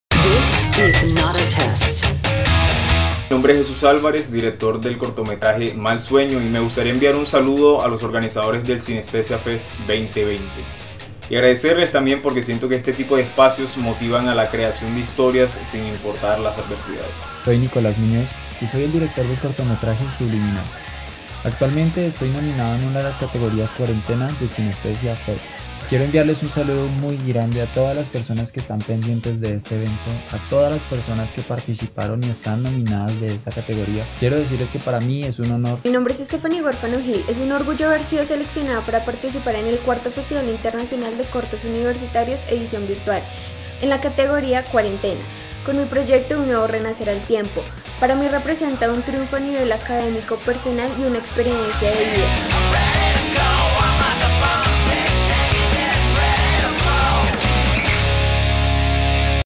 Cinestesia Fest 2020
¿Sabías que Cinestesia Fest nace en el año 2017 como una iniciativa de profesores de la Facultad de Ciencias de la Comunicación?